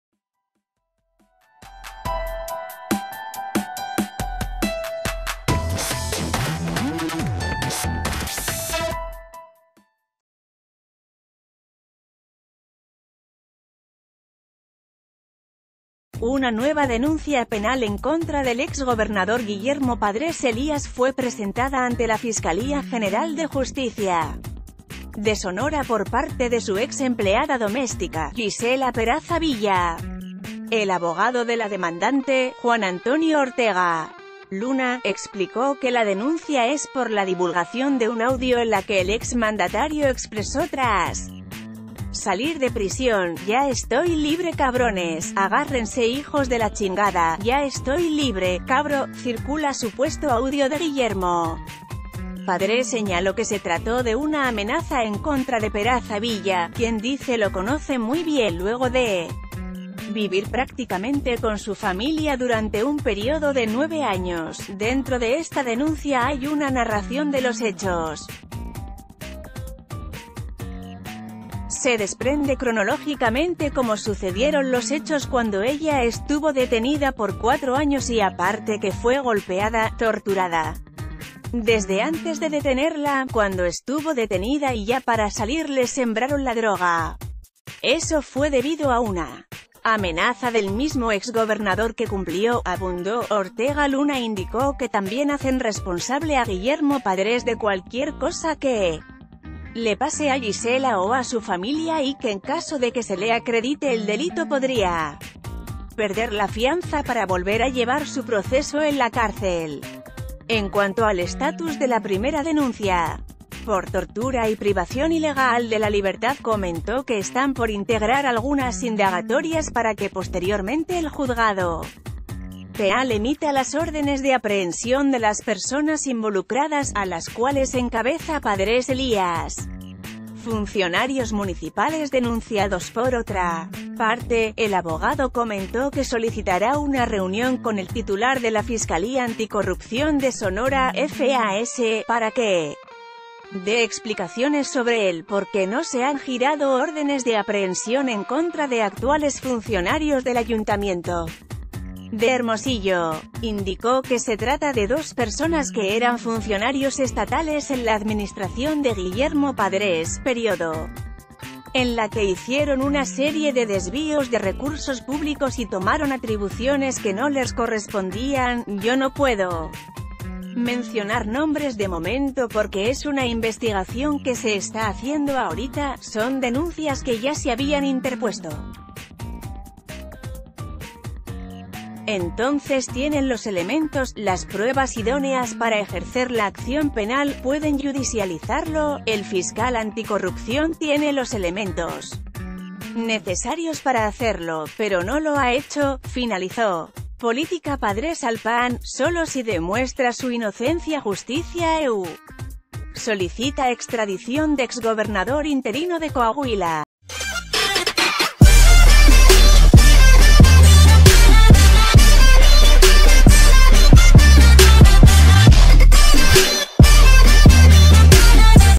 Denuncian a Guillermo Padrés por audio grabado después de salir de la cárcel
Una nueva denuncia penal en contra del ex gobernador Guillermo Padrés Elías fue presentada ante la Fiscalía General de Justicia de Sonora por parte de su ex empleada doméstica, Gisela Peraza Villa. El abogado de la demandante, Juan Antonio Ortega Luna, explicó que la denuncia es por la divulgación d...